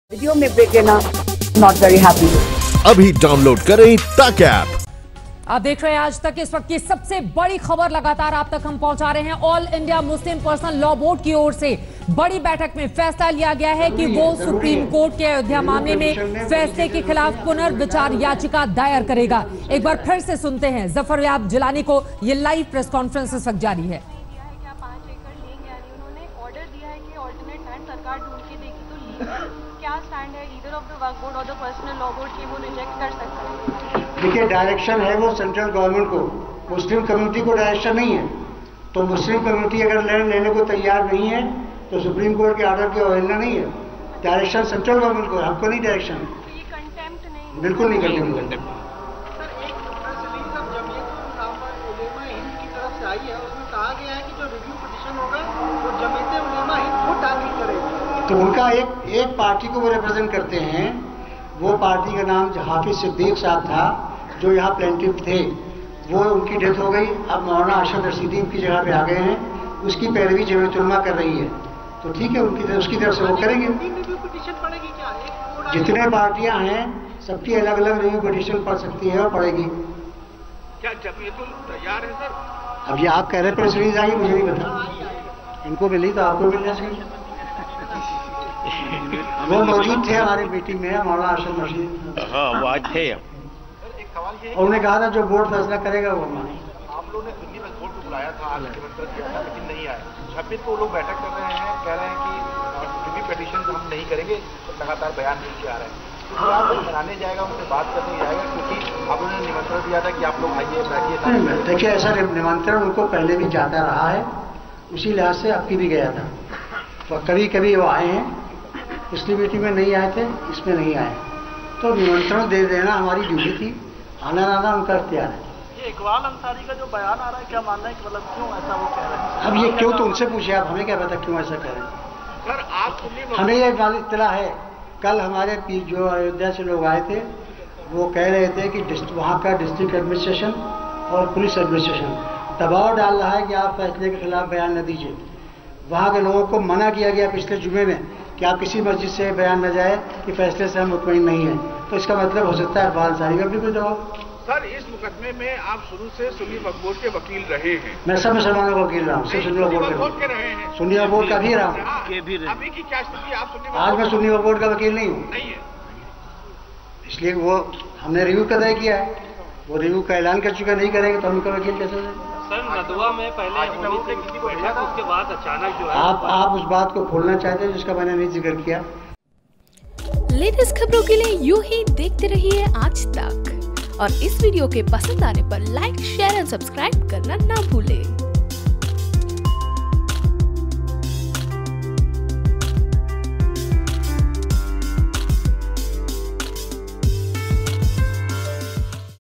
Ayodhya Verdict: AIMPLB का ऐलान, हमें वही जमीन चाहिए जिसके लिए लड़ाई लड़ी
ऑल इंडिया मुस्लिम पर्सनल ला बोर्ड यानी कि AIMPLB ने कहा है कि अयोध्या पर आए सुप्रीम कोर्ट के फैसले के खिलाफ वो पुनर्विचार याचिका करेगा. इसके अलावा AIMPLB ने कहा कि उसे मस्जिद के बदले दूसरी जगह पर दी जाने वाली 5 एकड़ जमीन मंजूर नहीं है. AIMPLB का कहना है कि वे दूसरी जमीन पाने के लिए अदालत नहीं गए थे, उन्हें वही जमीन चाहिए जहां पर बाबरी मस्जिद बनी थी.